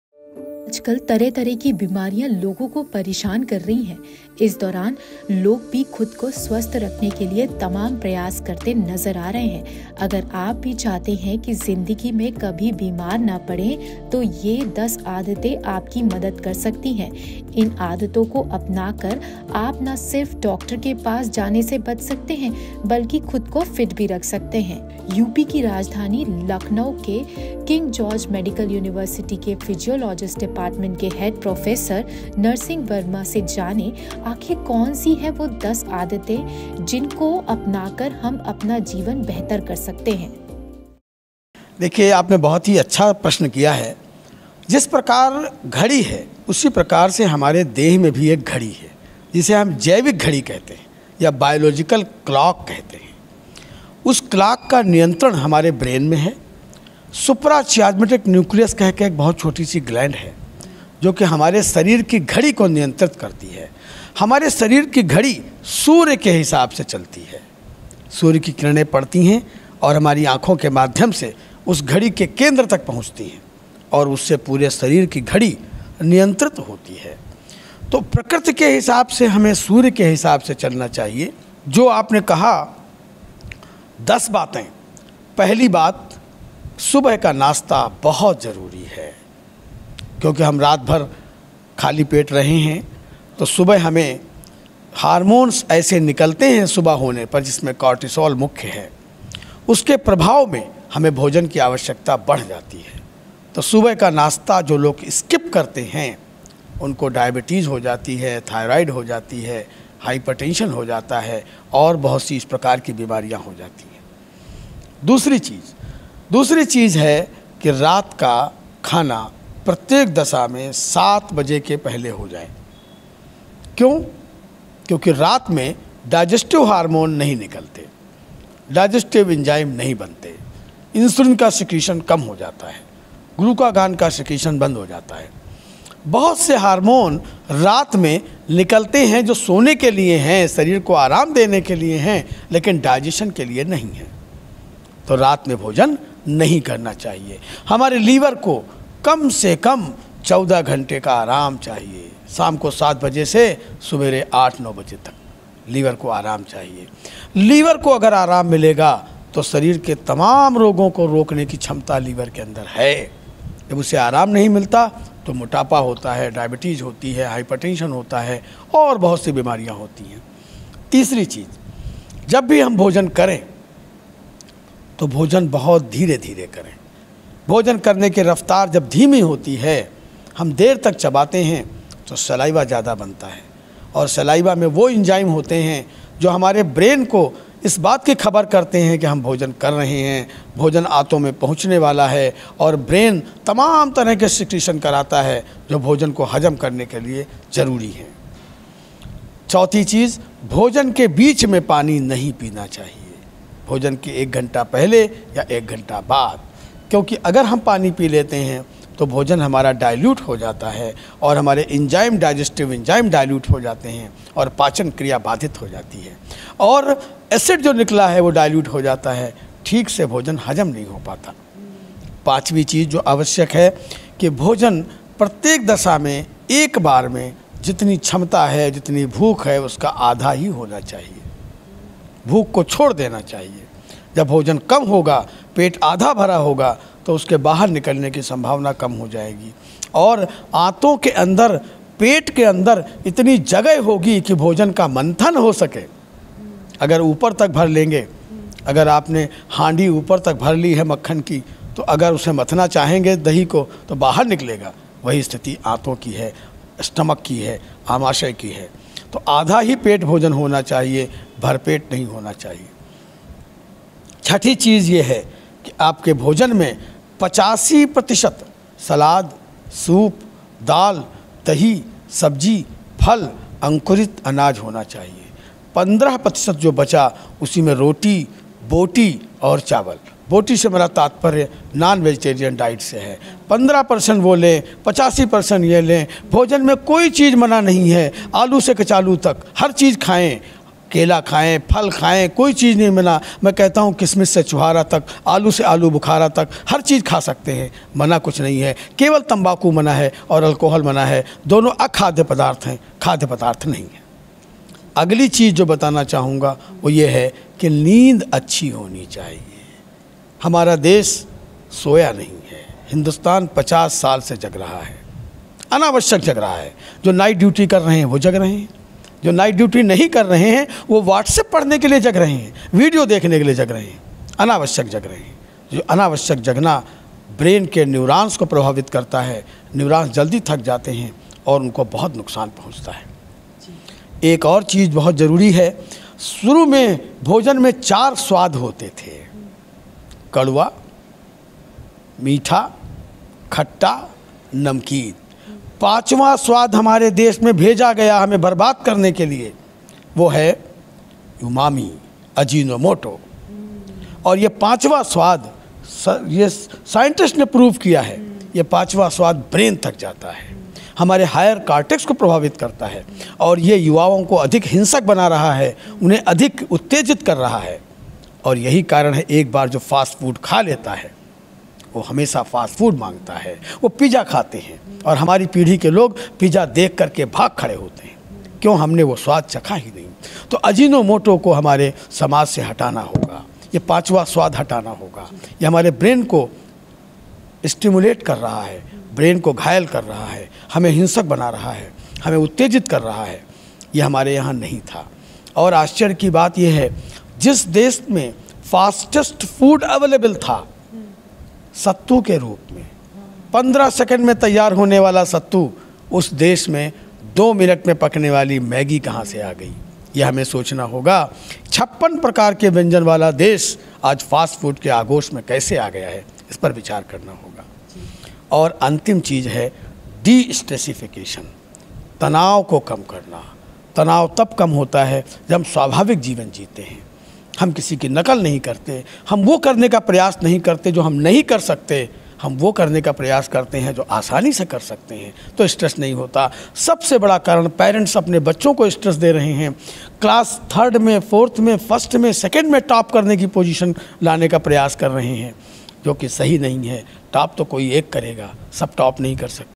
Health Tips: ये कुछ आदतें बदल देंगी आपकी ज़िन्दगी, हमेशा रखेगी फिट और पॉजिटिव!
Lucknow News:  आजकल तमाम बीमारियां लोगों को परेशान कर रही हैं. इस बीच आप ये 10 आदतें अपनाकर खुद को स्‍वस्‍थ रख सकते हैं. आइए जानें लखनऊ के किंग जॉर्ज मेडिकल यूनिवर्सिटी के फिजियोलॉजी डिपार्टमेंट के हेड प्रोफेसर नरसिंह वर्मा से टिप्‍स.
Lucknow News: Nowadays all diseases are troubling people. In the meantime, you can keep yourself healthy by adopting these 10 habits. Let's know tips from Professor Narsingh Verma, Head of the Department of Physiology, King George's Medical University, Lucknow.